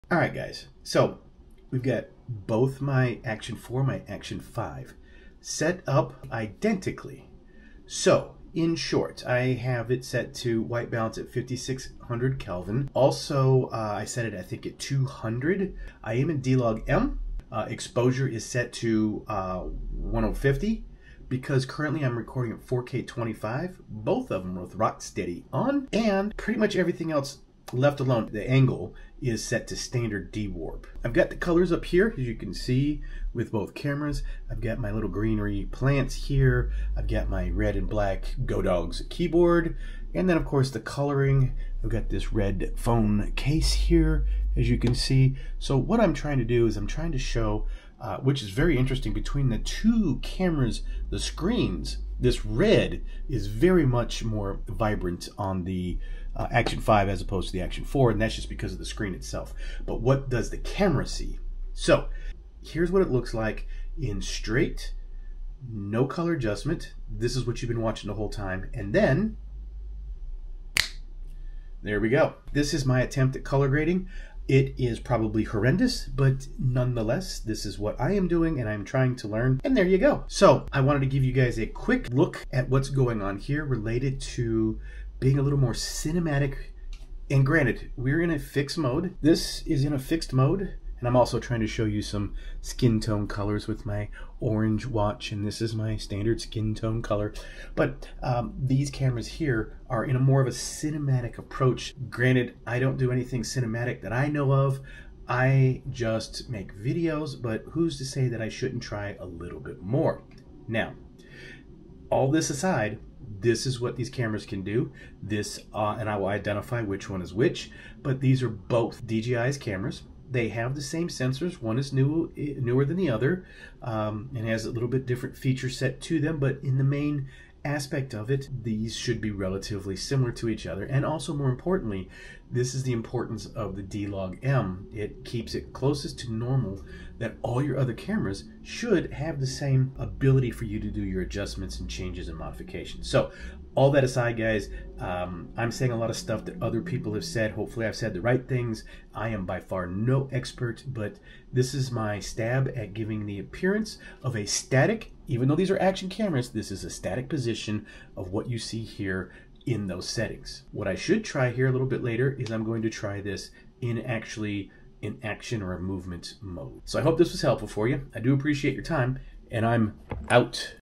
DJI Osmo Action 5 Pro, A Beginner's Guide to D Log M Settings and Color Grading
Thank you and you can find the product I am showing above here:

In this video, I set up DJI Action 4 and Action 5 cameras identically to test their performance and color grading capabilities. Both cameras are configured with a white balance of 5600 Kelvin, D Log M mode, and 4K25 resolution with Rocksteady on. I examine the screen differences, particularly in rendering vibrant reds and skin tones, and attempt to give a more cinematic feel despite being in a fixed mode. Action 5 seems to show a more vibrant red on its screen compared to Action 4. I provide a straightforward look without color adjustment and my first attempts at color grading, while highlighting the importance of D Log M for maintaining normalcy across devices. Although not an expert, I'm exploring the potential of these action cameras in a static setup, with plans to test movement modes in future videos.

00:00 Introduction and Camera Setup
00:45 Color Comparison Between Cameras
01:12 Color Grading Attempt
01:20 Cinematic Approach and Skin Tone Test
01:50 DJI Camera Features and D Log M Importance
02:40 Conclusion and Future Plans